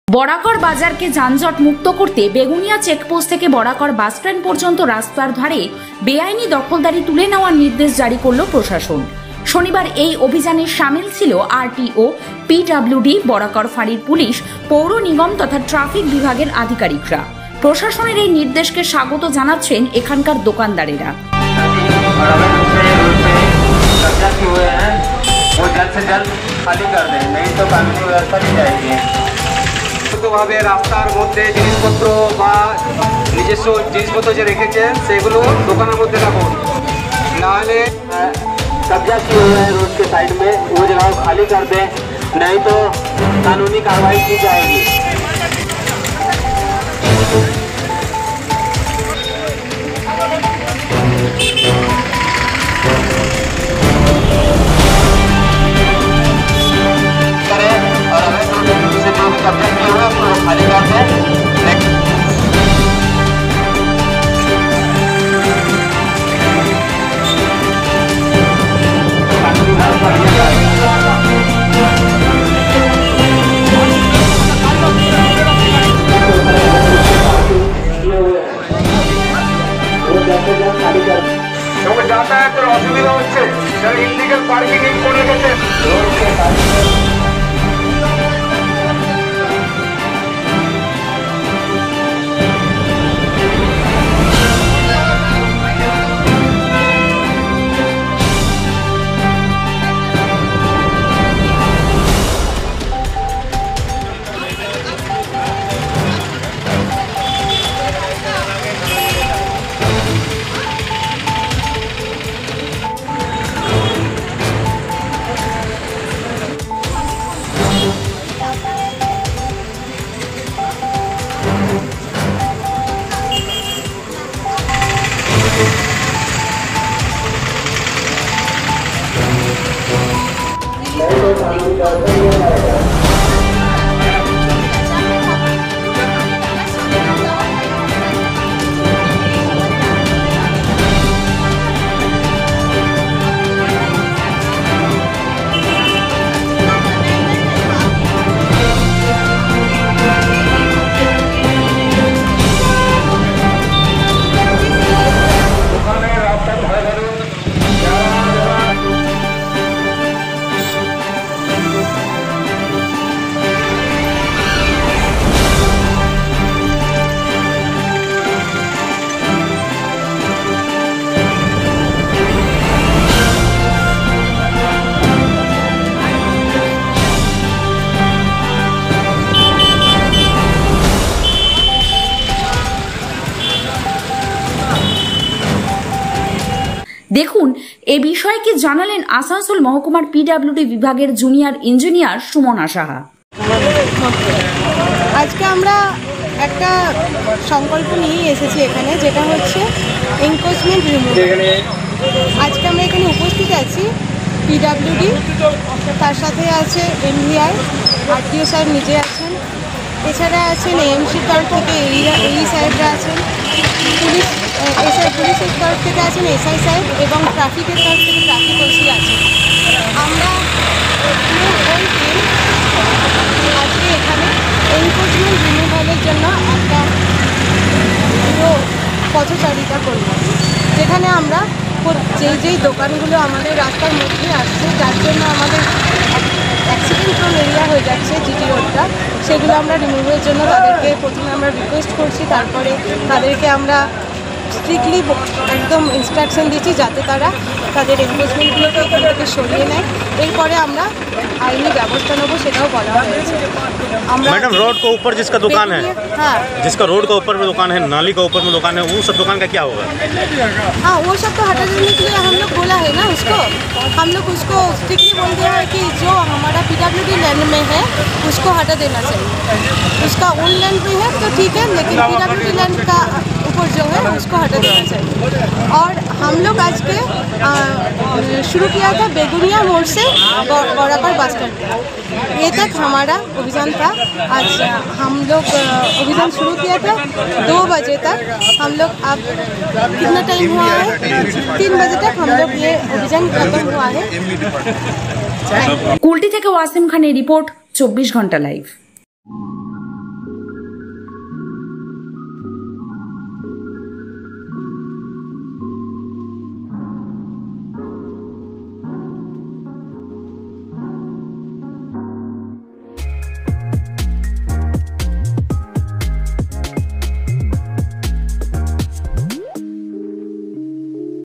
kulti News:বরাকর বাজারকে যানজট মুক্ত করতে বেআইনি দখলদারি তুলে নেওয়ার নির্দেশ জারি করলো প্রশাসন
বরাকর বাজারকে যানজট মুক্ত করতে বেআইনি দখলদারি তুলে নেওয়ার নির্দেশ জারি করলো প্রশাসন